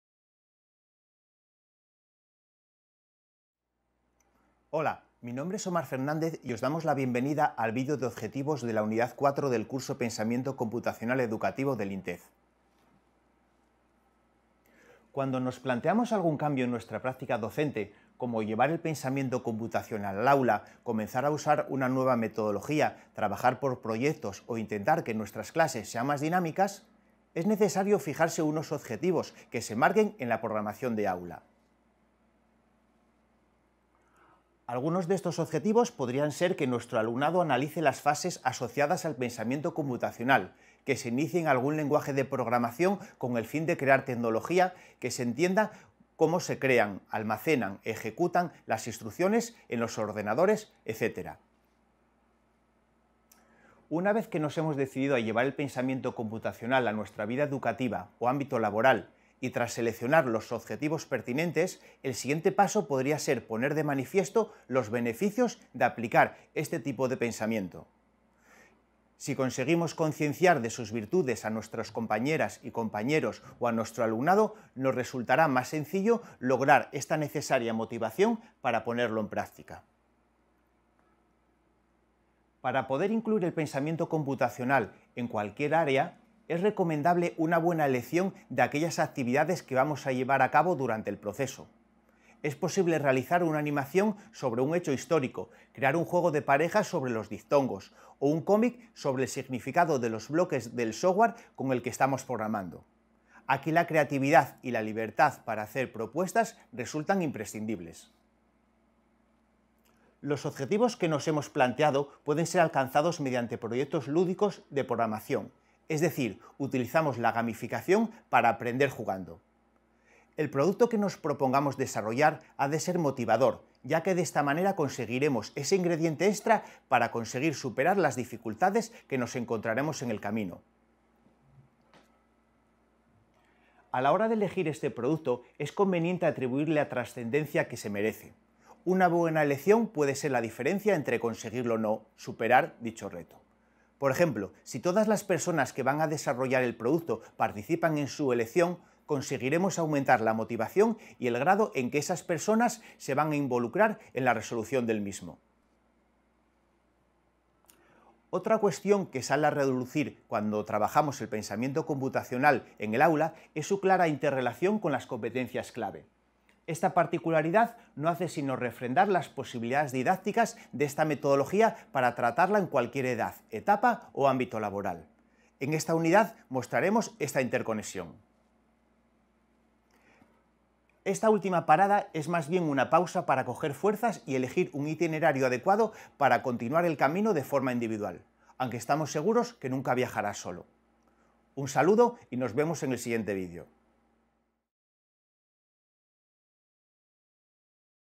Vídeo 4.1. "Integración del Pensamiento Computacional en el aula" - Objetivos
Vídeo 1 de la Unidad 4 del MOOC de AprendeINTEF "Pensamiento computacional educativo" MoocBot. Para más información, visita